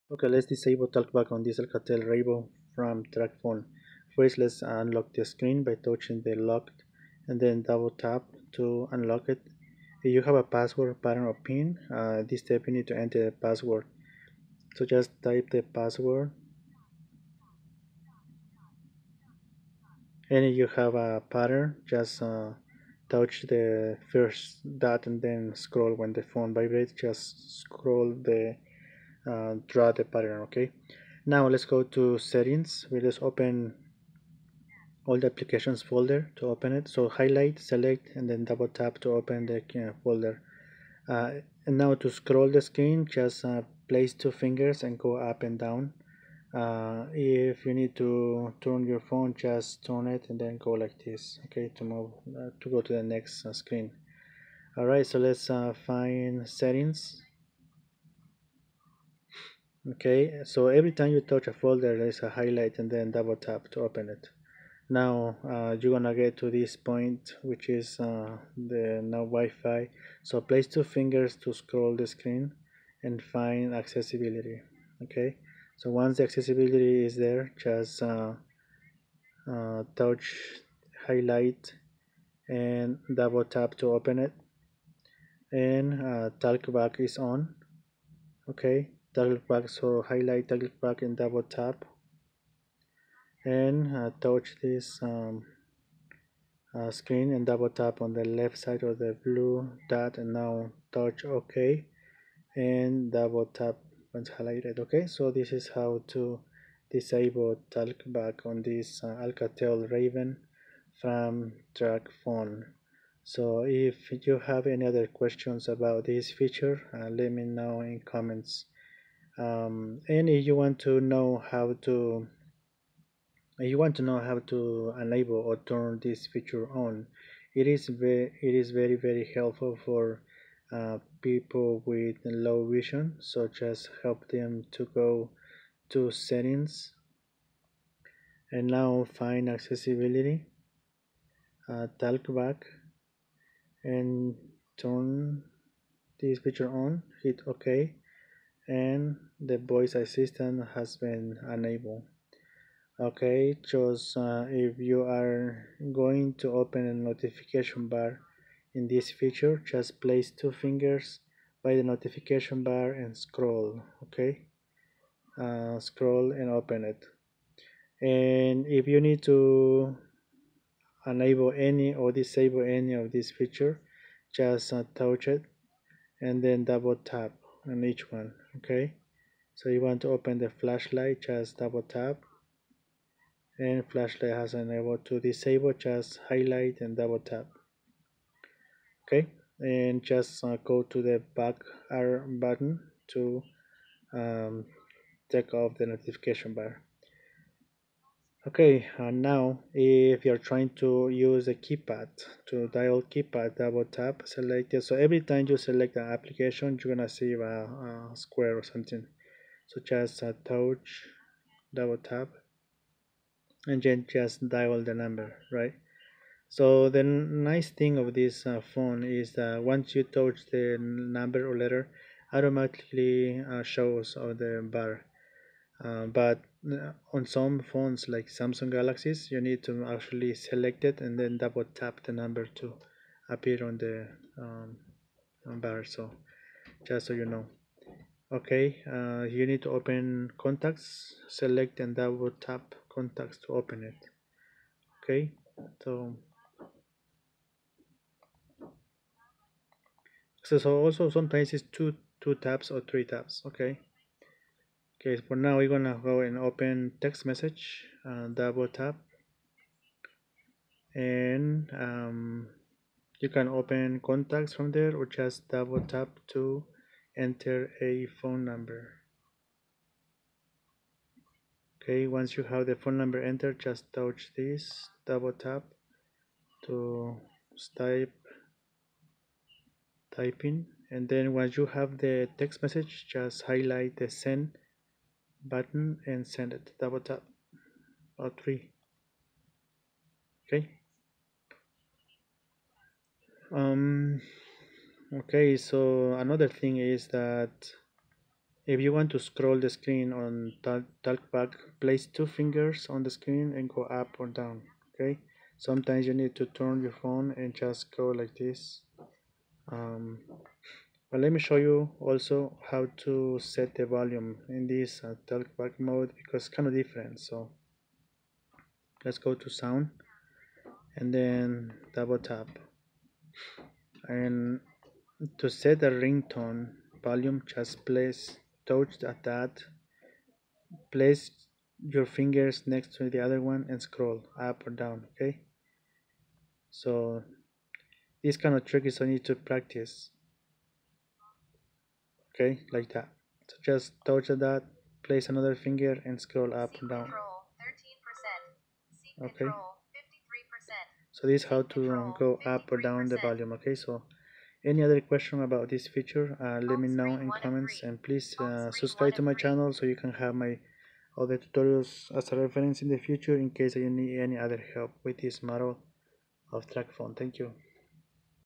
Turns off voice talkback on Alcatel RAVEN LTE TRACFONE Model A574BL
There is a voice reading out the aplications on the phone, talkback is on, also known as voice assistant for Alcatel RAVEN LTE TRACFONE Model A574BL